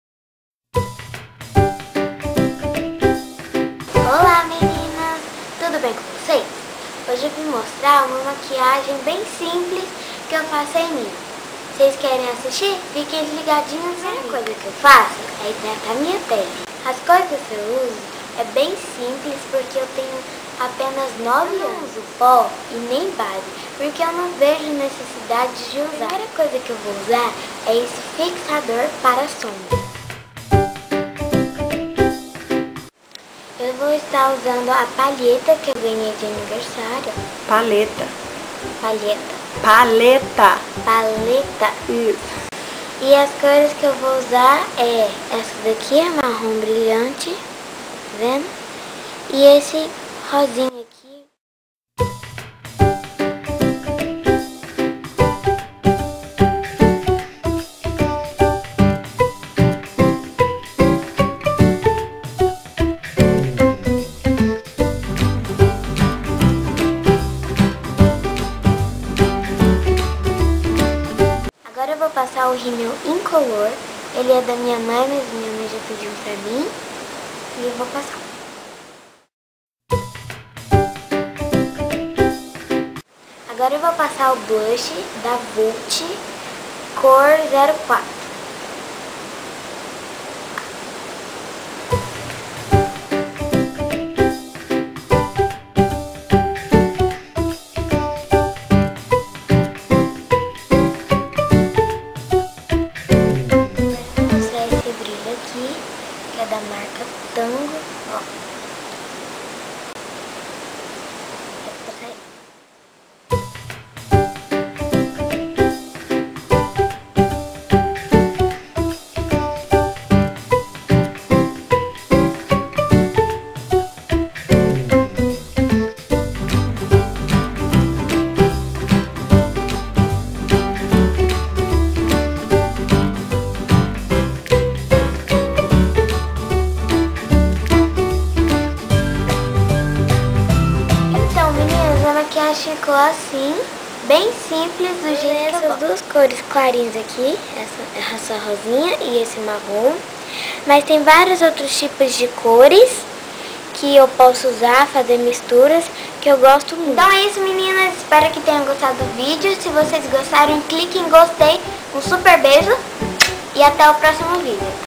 maquiagem bem simples
maquiagem bem simples..Quando quero maquiagem mais elaborada minha mãe que faz em mim..mais é dificilmente eu usar maquiagens mais fortes.